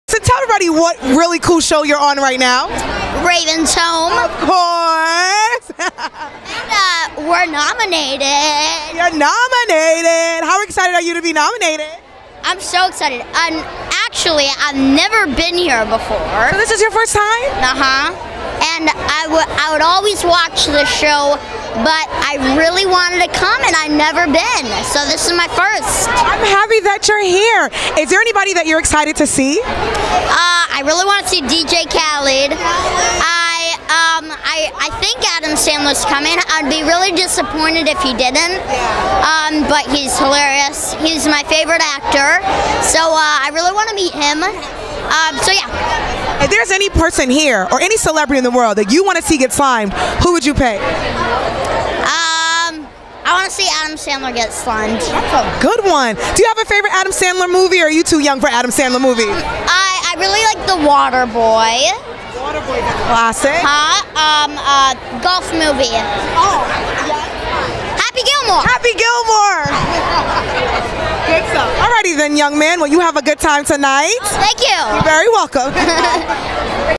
Jason Maybaum | Kids Choice Awards 2019
Host Nakia Monet chats with Ravens Home star Jason Maybaum at the 2019 KCA's